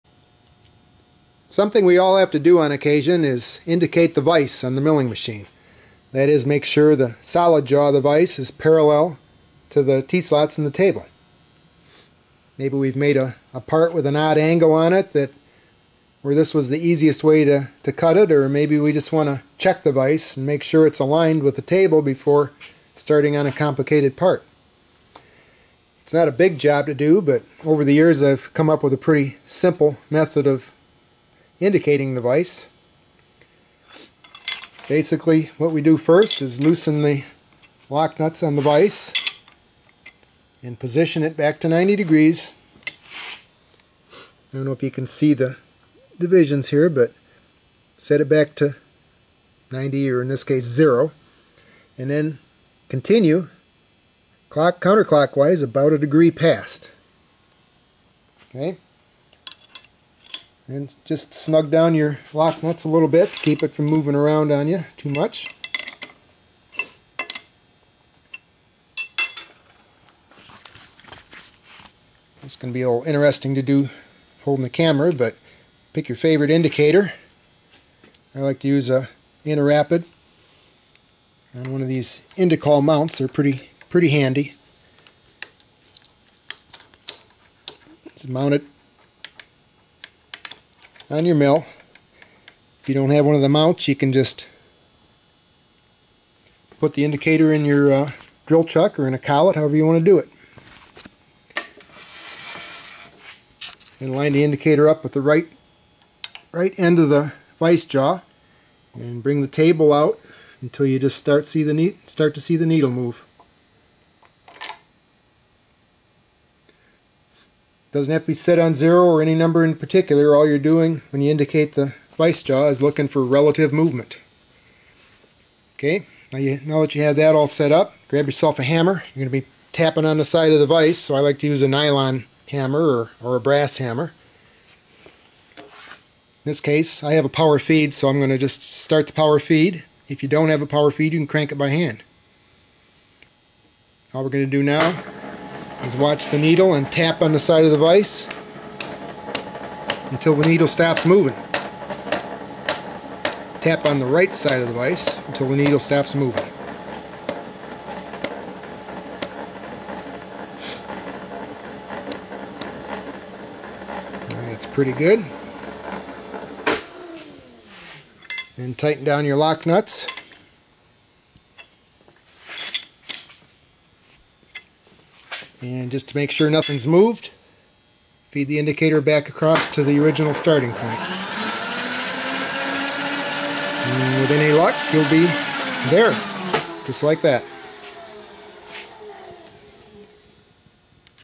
Indicate the vise on a milling machine in just one pass
Time spent on machine set-up is time lost making chips. Here's a quick and simple method to indicate the vise in just one pass and in less than a minute.